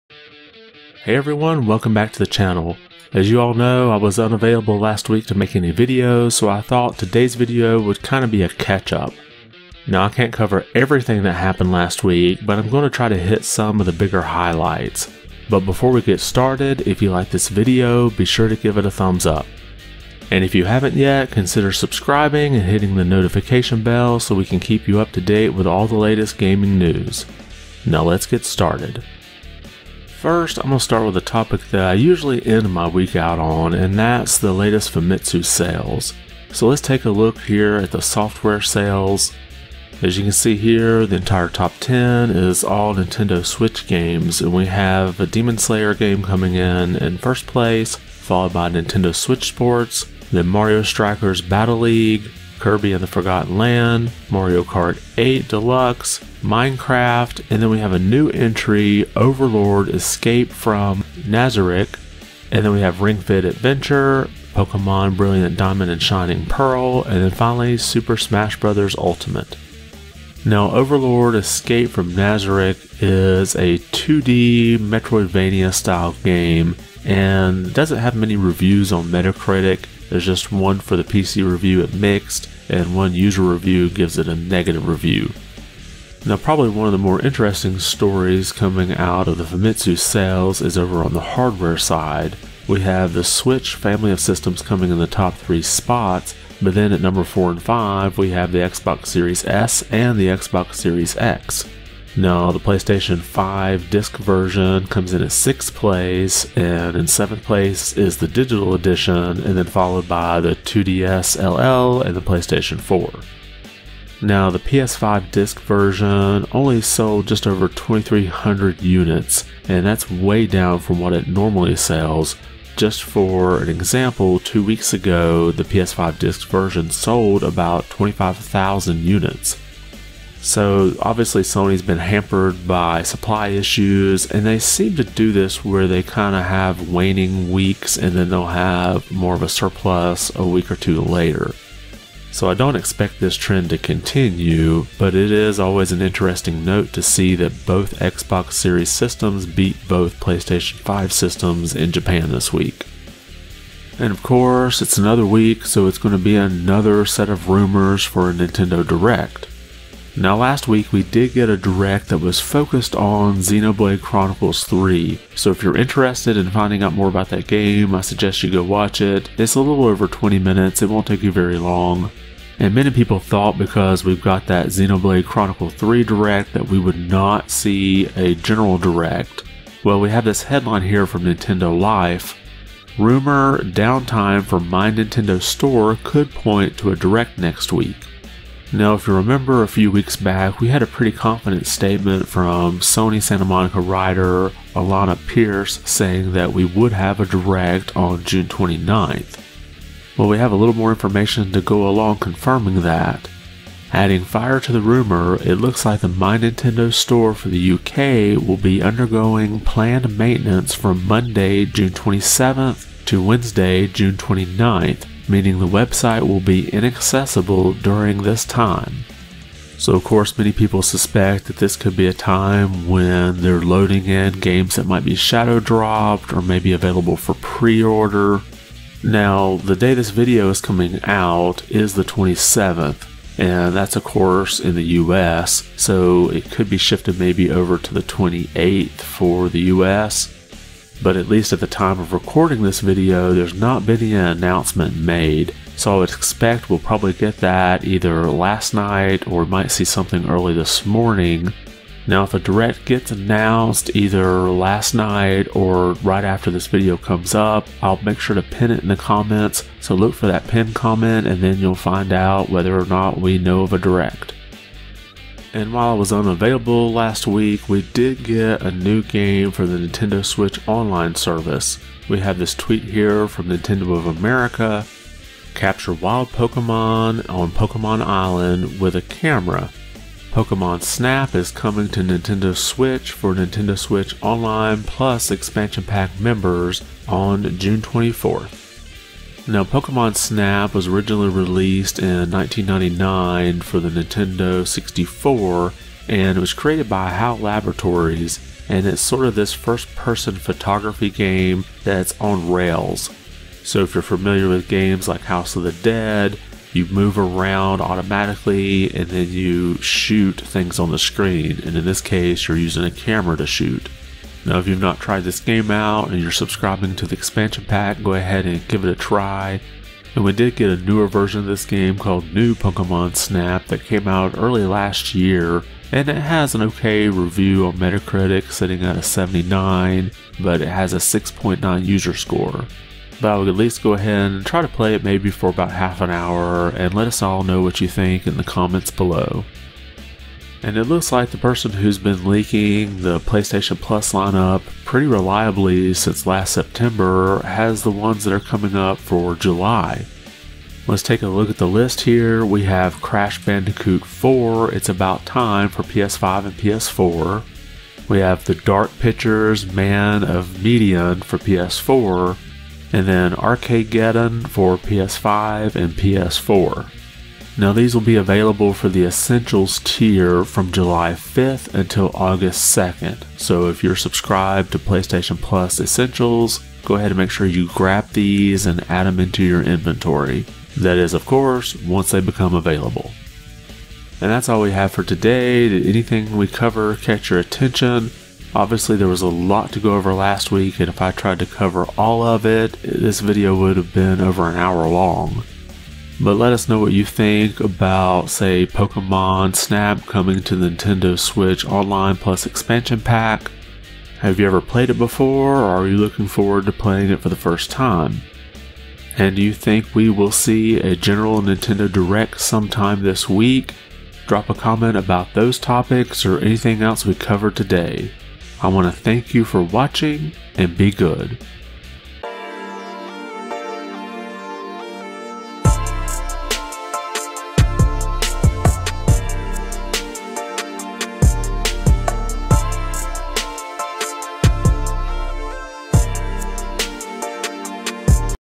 Latest Famitsu Sales, Nintendo Direct Watch, Pokemon Snap 64, July PS Plus Games
We take a look at the latest Famitsu Sales. More info pointing to a Nintendo Direct this week. Pokémon Snap added to the Switch Online Expansion Pack. PlayStation Plus Essential games for July.

Chapters
0:00 Opening
0:29 Latest Famitsu Sales
2:33 Nintendo Direct Watch
4:24 Pokémon Snap 64
5:39 July PS Plus Games